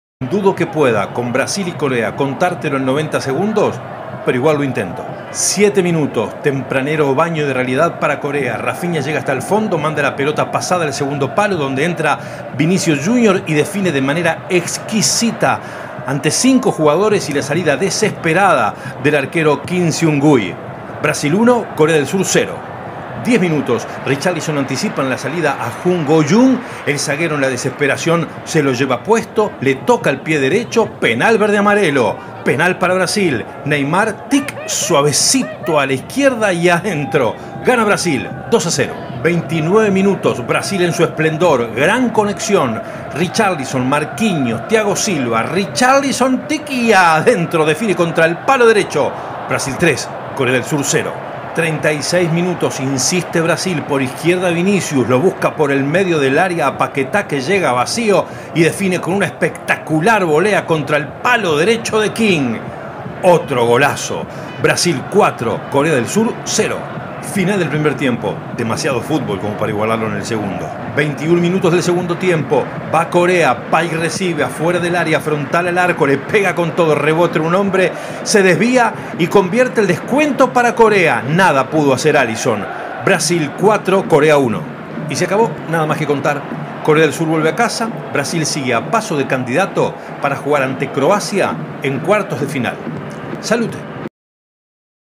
🇧🇷 BRASIL 4 🇰🇷 KOREA DEL SUR ⚽️ 90' en 90" ⏱️ Qatar2022🏆
Resumen del partido por octavos de final del Mundial Qatar 2022 de Pepe Mansilla en 90 segundos.